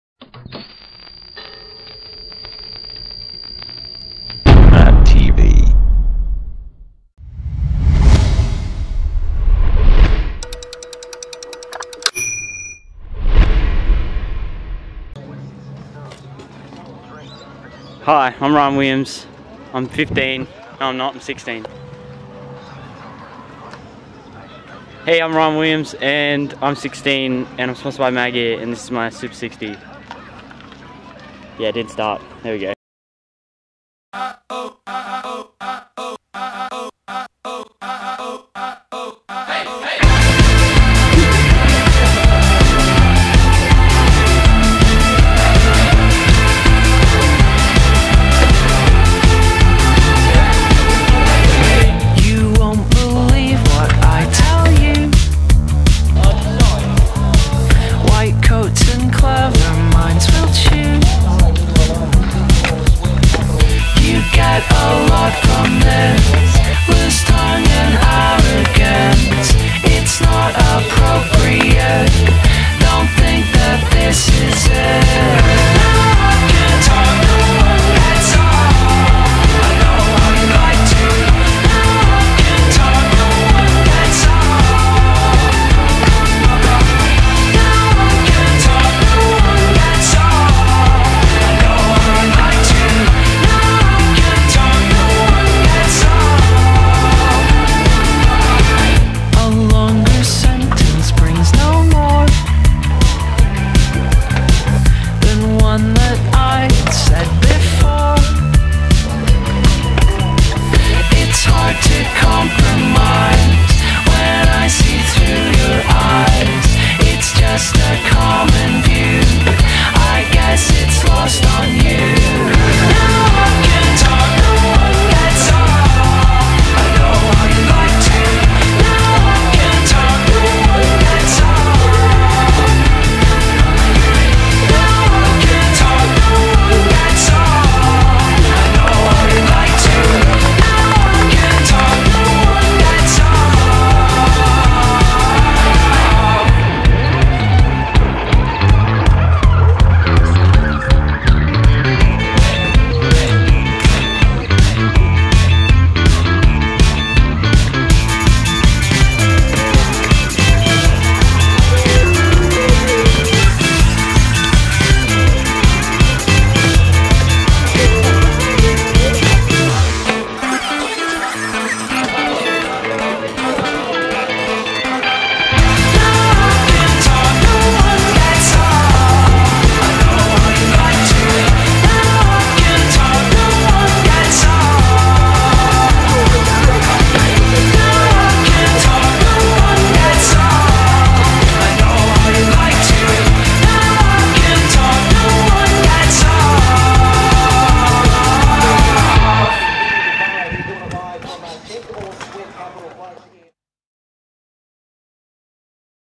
Ryan Williams Super Sixty
Ryan Williams Shredding His Local Park,
Filmed on Stop One of the East Coast Tour :)